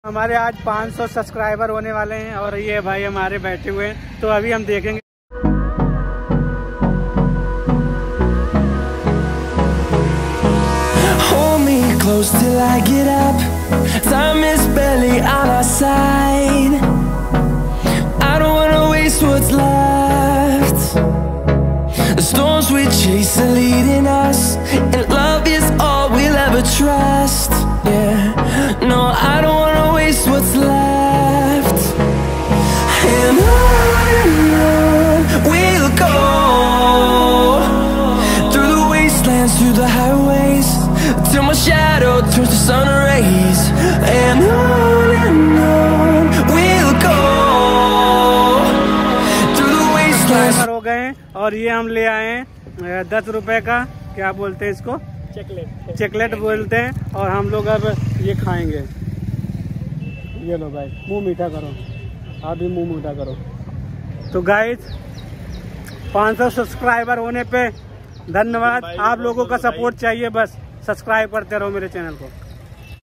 500 Subscribers Celebration | Thanks Family | 500 Subscriber on youtube | Life 8bit Vlogs
I am very happy because of today I got 500 Subscribers on youtube.
and We have done small celebration. thanks you everyone.

2nd Camera Canon 90D
50MM LENS
CAMERA TRIPOD